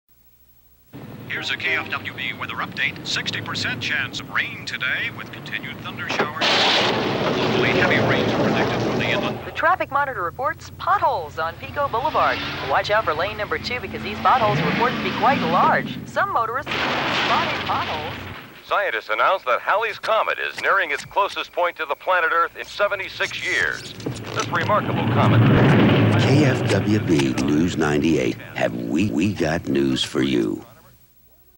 KFWB-AM Los Angeles 1984
Here's a 1984 spot for KFWB Los Angeles, then owned by Group W. See more 1980s TV spots for radio at the syndinet channel on YouTube.